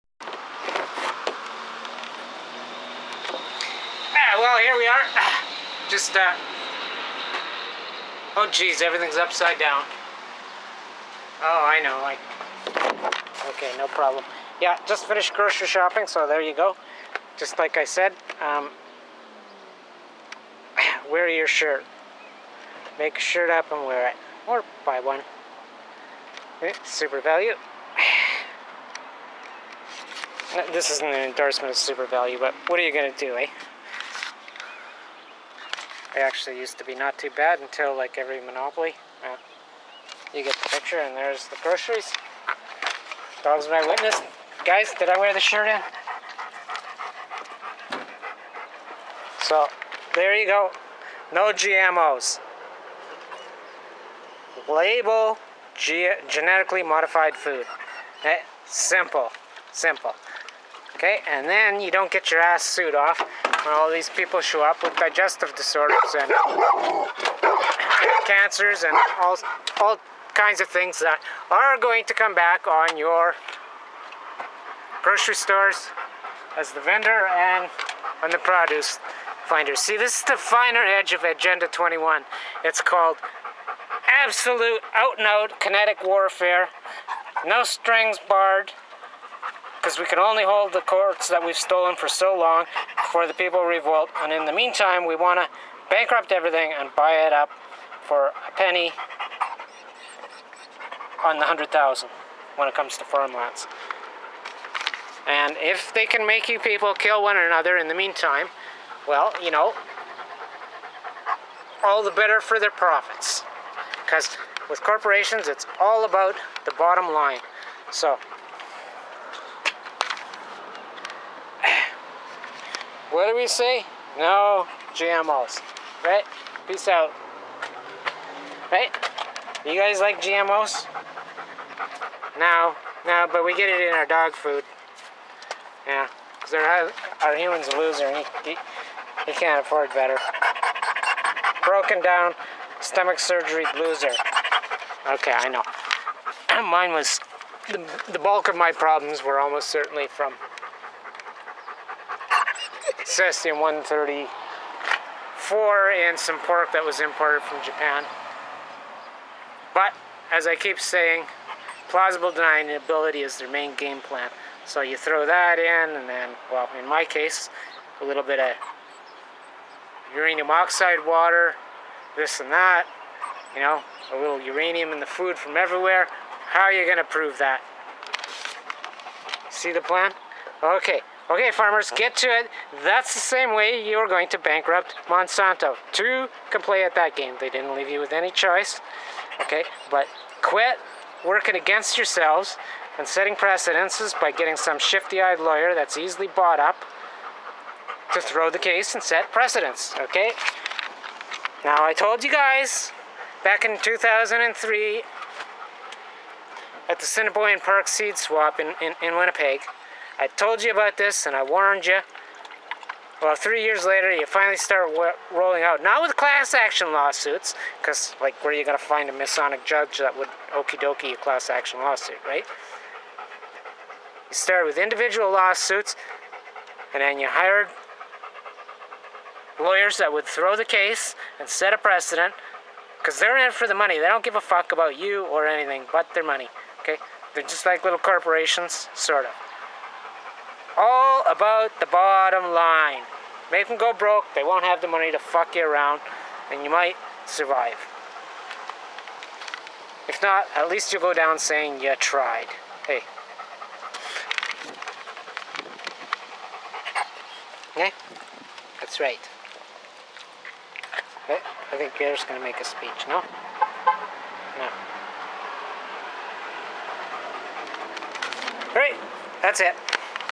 Grocery Shopping for kNOw GMO "homemade shirts"
Grocery Shopping for NO GMO
label genetically modified foods
sign a lable GMO petiion for the Green Party to take to Canadian parliment